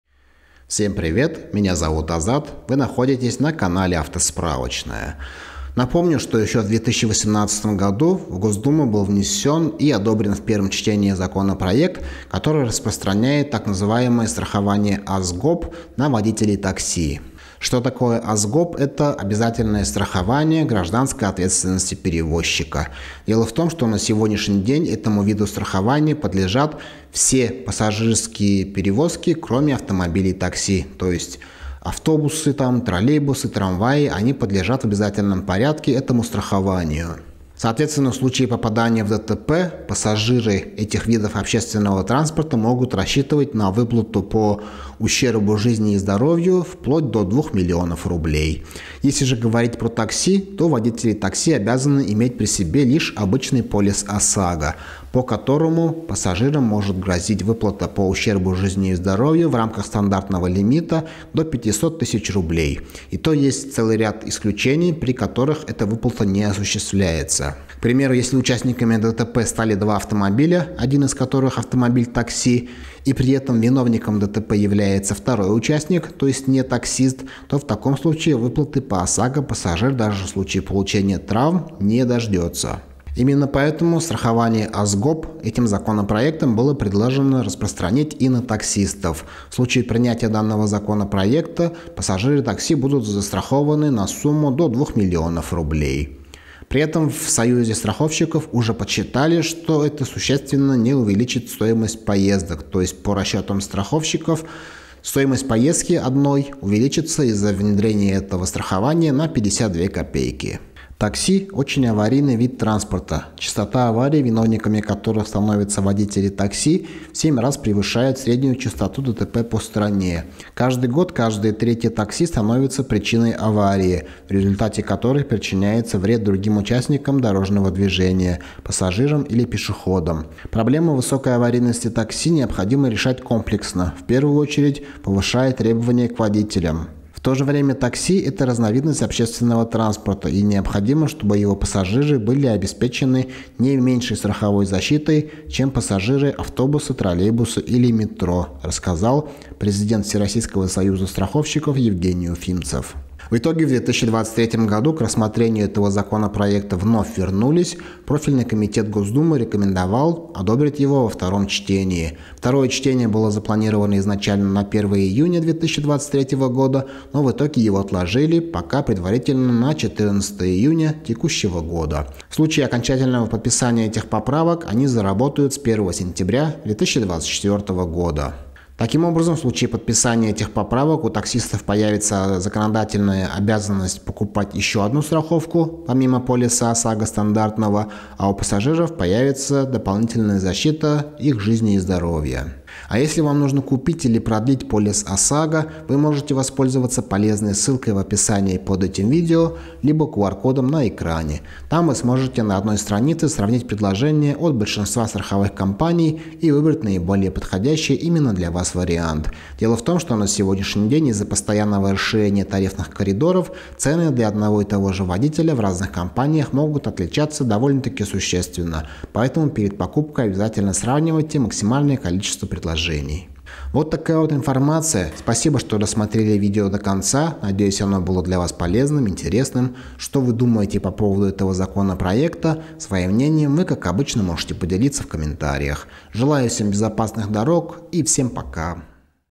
Страховку для пассажиров такси рассмотрят во 2 чтении
Госдума рассмотрит во втором чтении поправки о страховании пассажиров такси на 2 миллиона рублей.